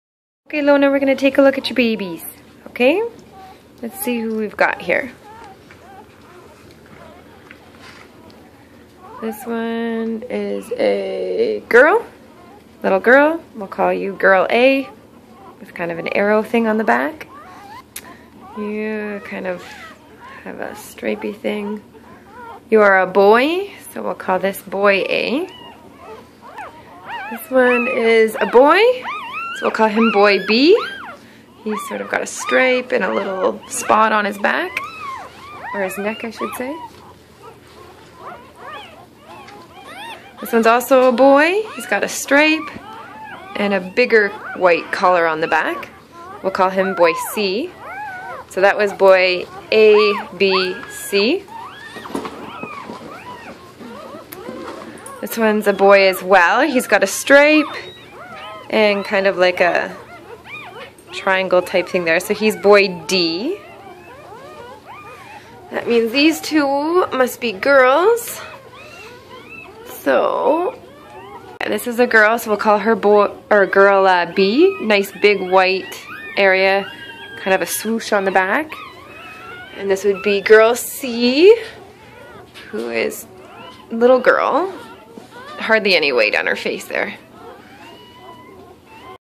Lona had her 2016 litter of pups - lets introduce them!
Lona had 7 puppies on June 22. Here they are 1 day old and we are introducing them for the first time. For now we are calling the puppy A, B, C, D, etc.